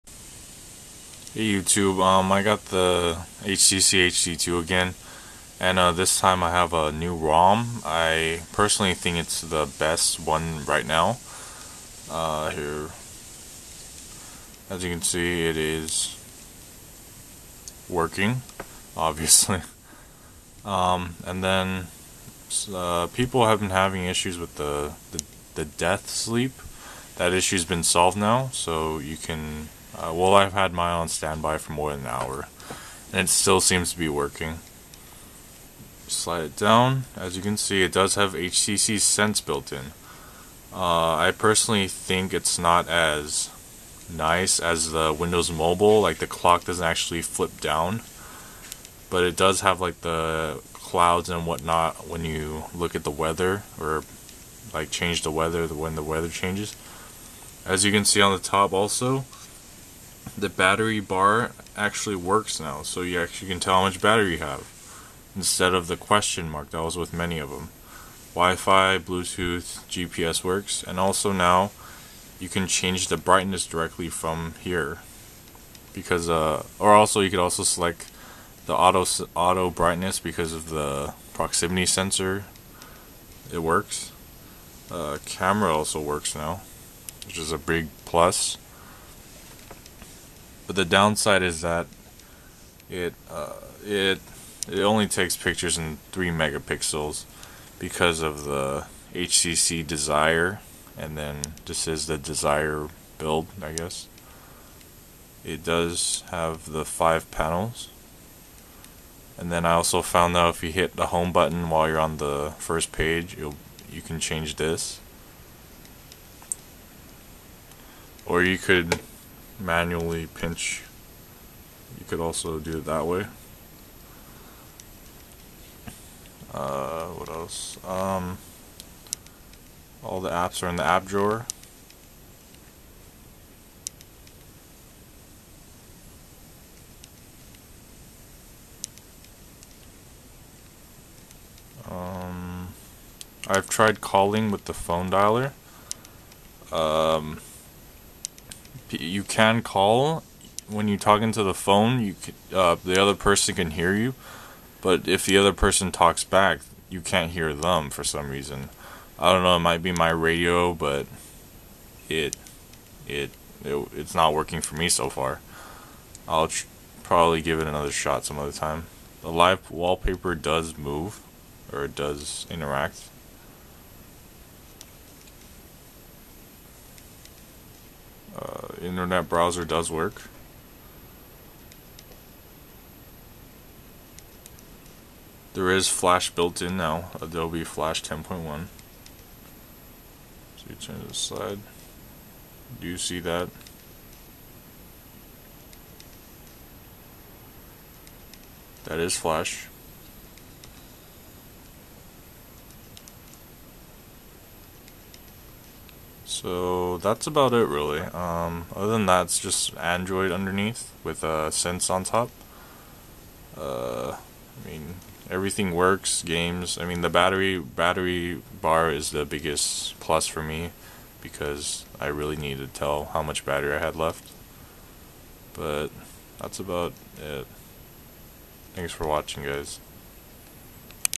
HTC HD2 Android with Sense v5 (Working Download link)
*You do not need Hardspl for android.. but if you are going to flash a custom rom, you need to!*
Hi all,
Im demonstrating the latatest & most stable build of android + htc sense in which (almost) everything works!

Okay im tired of answer "how do i get this to work questions" Heres the solution:
1. just put the files on the root of the sd card (when you plug it in the first window that pops up is the "root")
2. run clrcad.. nothing supposed to happen since its running in the background
3. Run Haret.exe
4. Enjoy

Thanks to gamesquare for uploading and polishing this build!

Questions or comments? post below.